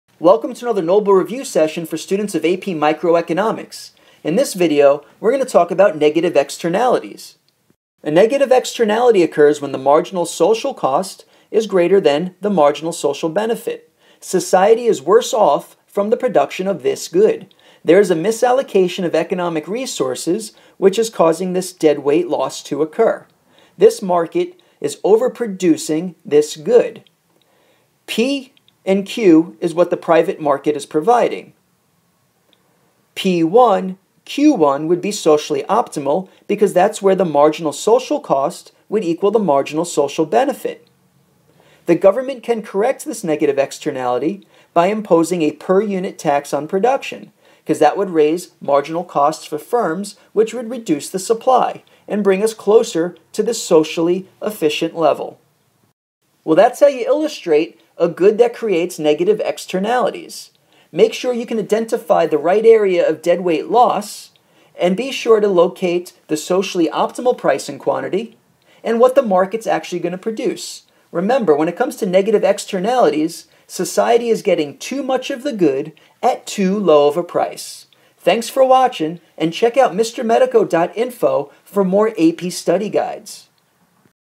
Negative Externalities - AP Microeconomics
How to graph negative externalities for the AP Microeconomics exam. This video also shows you where to place the deadweight loss and how the government can correct the externality.